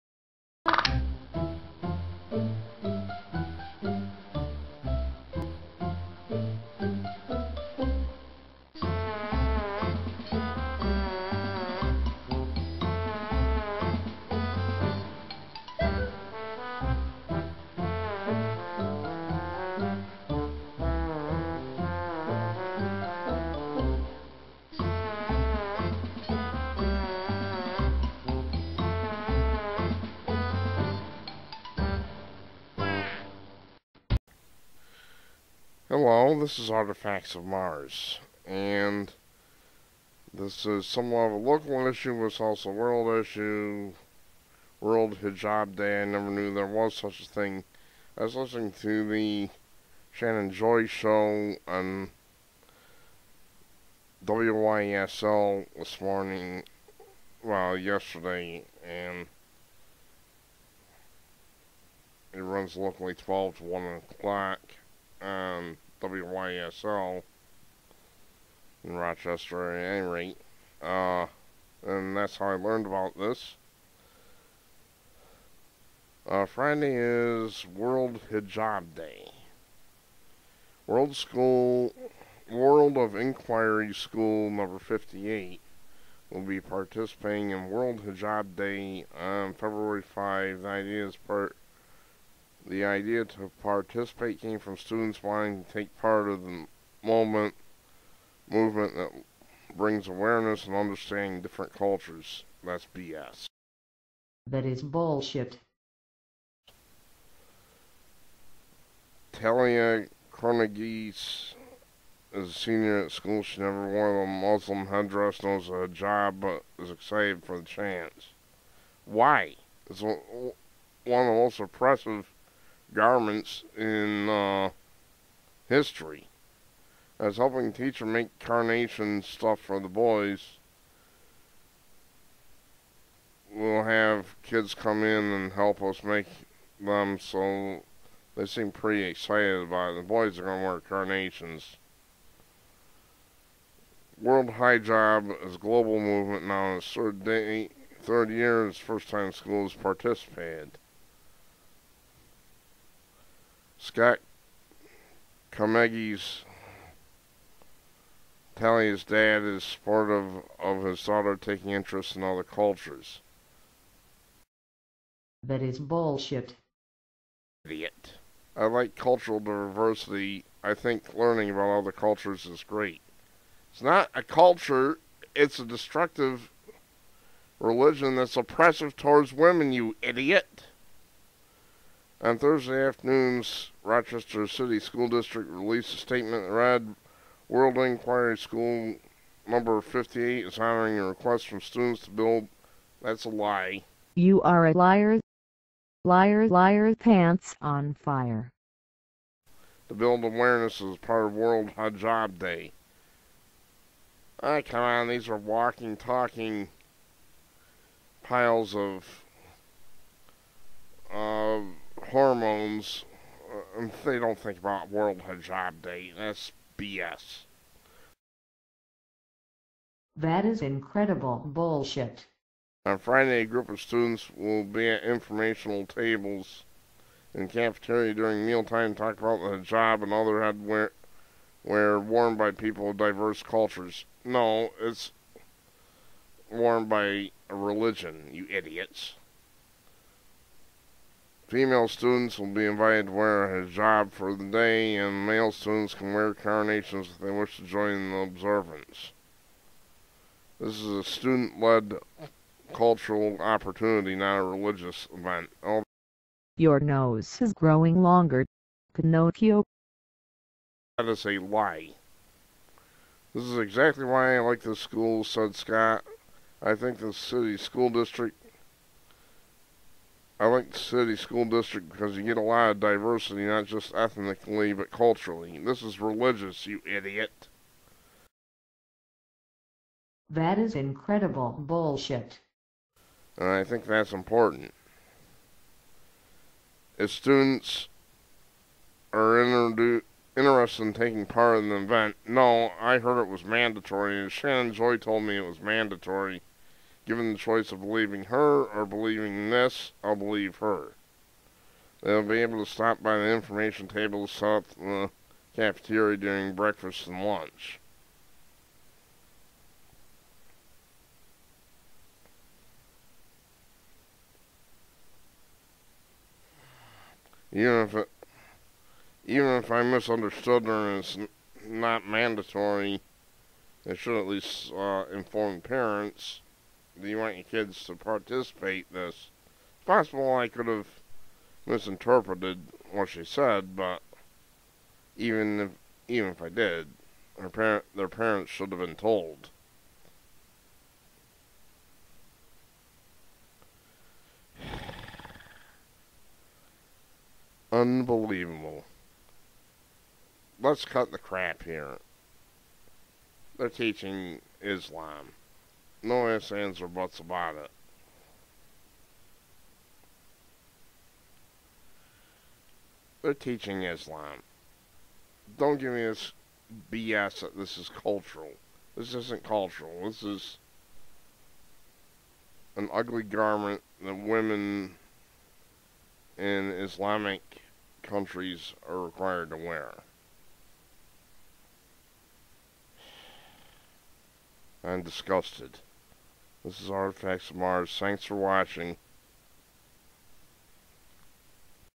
Rochester school makes girls wear Hijab for day to honor Islam!
Rochester school #58 is having female students wear the Hijab in honor of world Hijab day, angering many parents. It seems that these parents were not told.
   The article claims that this was thought up by the students. This is a lie. I have had it confirmed that it was thought up by teachers and administrators, not students. Kids are walking towers of hormones and don't think about things like this.